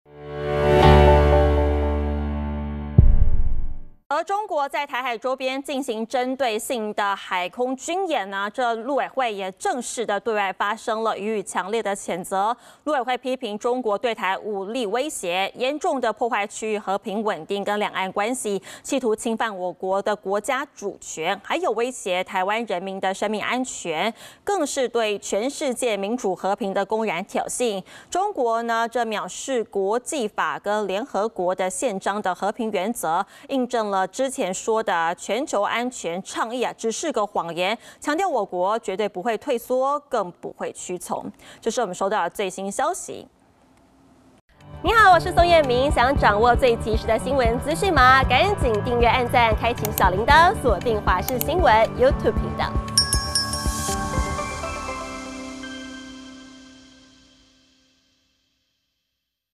陸委會批中武力威脅 嚴重破壞區域和平穩定｜華視新聞 20241014
中國在台海周邊進行針對性的海空軍演，陸委會正式對外發聲，予以強烈譴責！